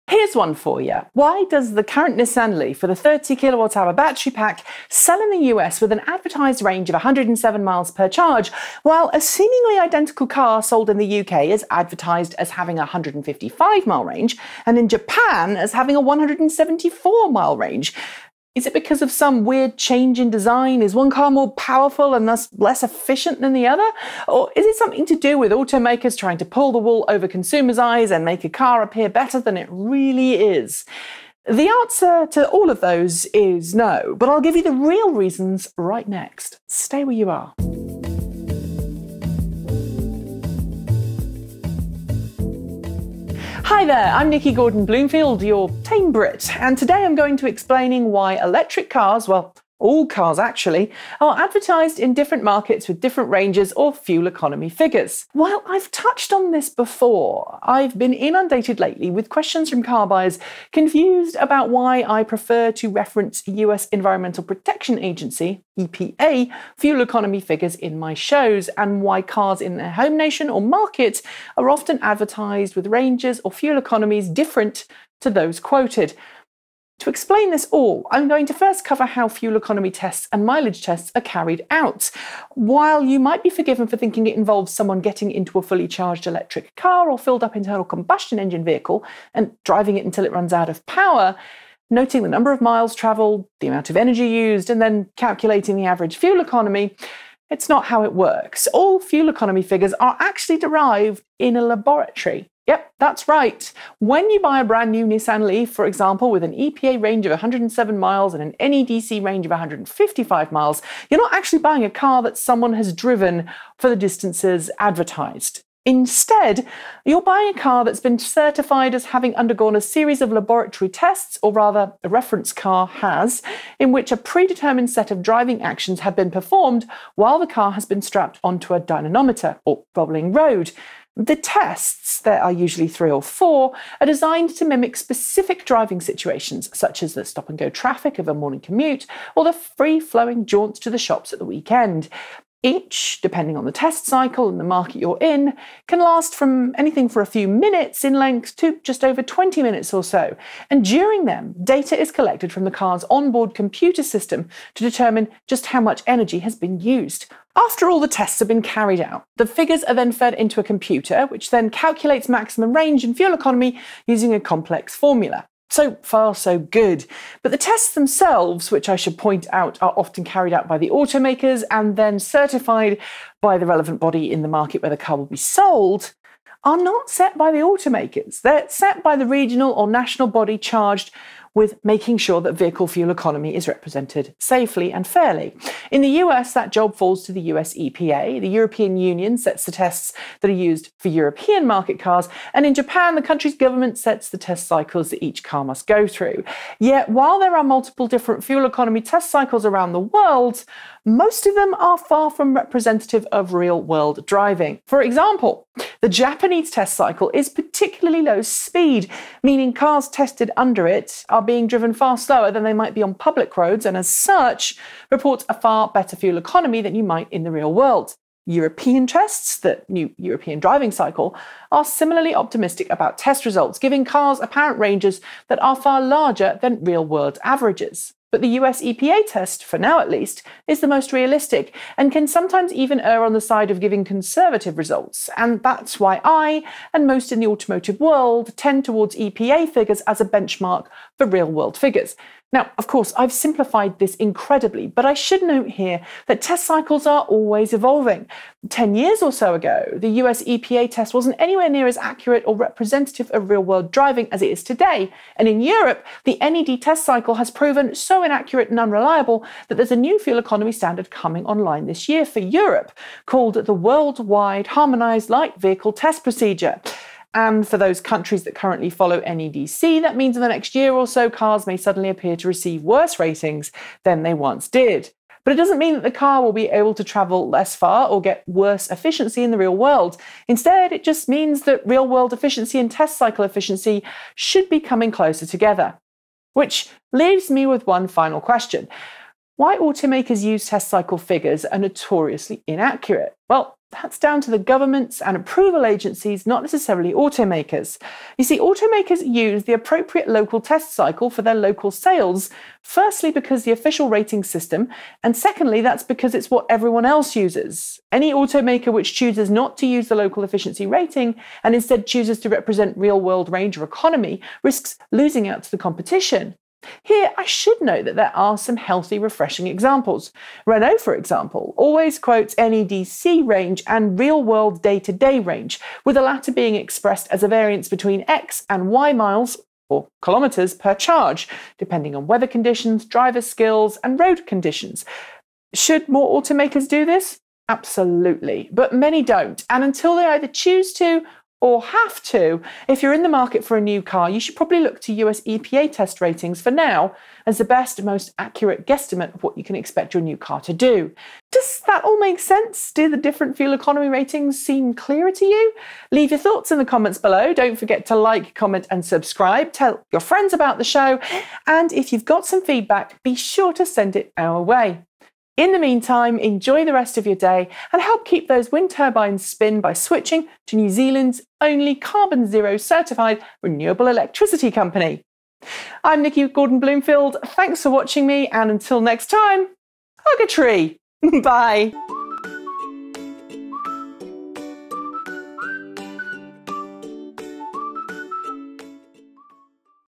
Electric Range Testing Explained, US EPA, European NEDC or Japan
Which international standard is best used?  The USA EPA is the most honest.  Check out all NZ Electric Vehicle Ranges